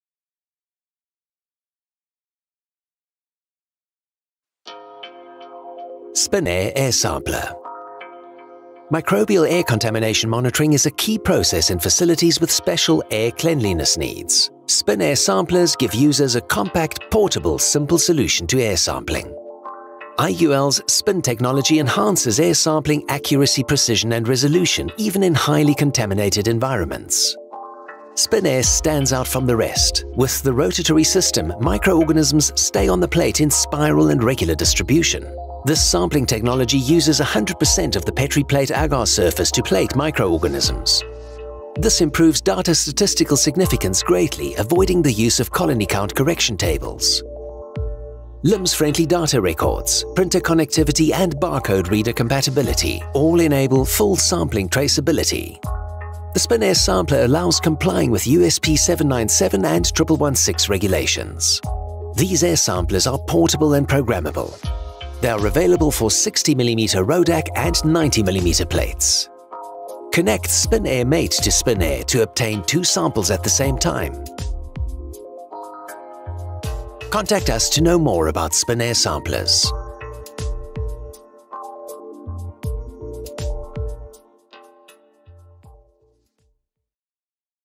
Microbial Air Sampler - Air sampling and environmental monitoring using Spin Air
Microbial air environmental monitoring is a key process in facilities with special air cleanliness needs. Spin Air samplers give users a compact, portable, simple solution to air sampling. Neutec Group’s microbial air samplers and microbial air sampling equipment are ideal for pharmaceutical, food, and beverage applications.